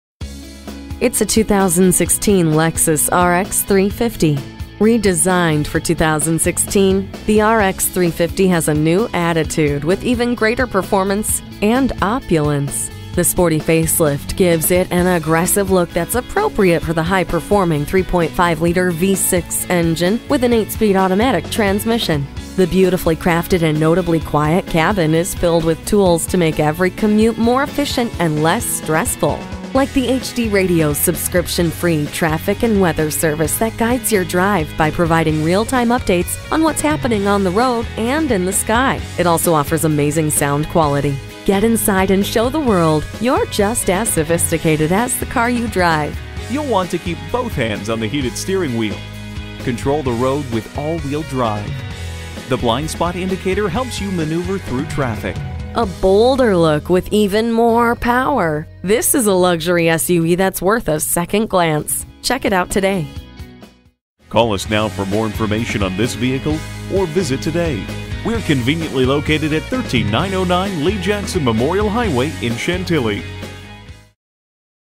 New 2016 Lexus RX 350 Chantilly Dale City VA DC, MD RXGC16006 - SOLD
Year: 2016
Make: Lexus
Model: RX 350
Trim: AWD 4dr
Engine: L 6 Cyl.
Color: Caviar
Interior: Black
Stock #: RXGC16006
VIN: 2T2BZMCA5GC016006
Packages P 2Q All Weather Floor Mats with Cargo Mat P Z1 Cargo Net,Cargo Mat,Wheel Locks & Key Glove Accessories F BD Blind Spot Monitor w/Rear-Cross Traffic Alert F FV 20 Gray Metallic Alloy Wheels w/Machined Finish F GL Lexus Safety System+ F HF Touch Free Power Rear Door F HS Heated and Ventilated Front Seats F NW Navigation System with 12.3 multimedia display Lexus Enform Destinations (1-year trial subscription included), Lexus Enform App Suite (Complimentary) F PA Intuitive Parking Assist F PM Premium Package Includes: Leather Trim Seats, Power-folding Outside Mirrors, Driver's Seat/Steering Wheel/Mirror Memory, Rain-sensing Wipers, Aluminum Roof Rails F SR Moonroof F WN Espresso Walnut Interior Wood Trim F WR Heated Leather Steering Wheel P 3T Cargo Cross Bars

Address:
13909 Lee Jackson Memorial Hwy